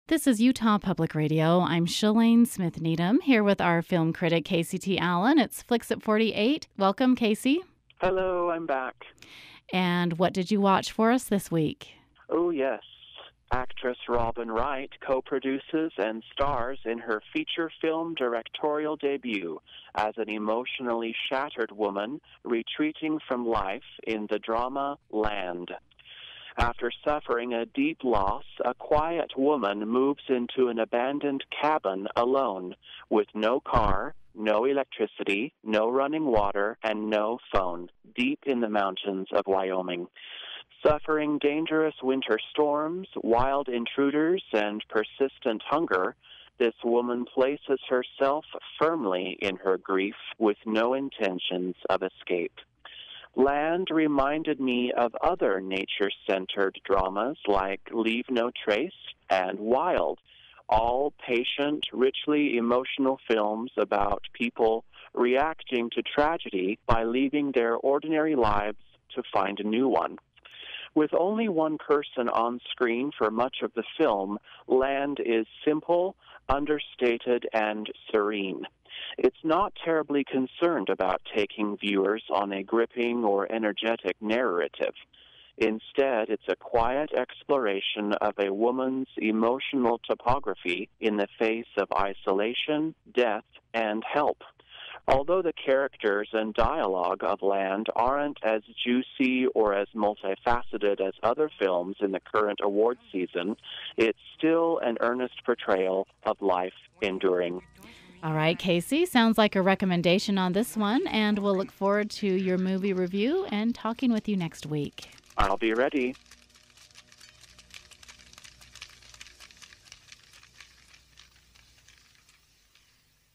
'Land' Review With Casey
Actress Robin Wright (Wonder Woman 1984, 2020) co-produces and stars in her feature film directorial debut as an emotionally shattered woman retreating from life in the drama Land. After suffering a deep loss, a quiet woman moves into an abandoned cabin alone with no car, no electricity, no running water, and no phone deep in the mountains of Wyoming. Suffering dangerous winter storms, wild intruders, and persistent hunger, this woman places herself firmly in her grief with no intentions of escape.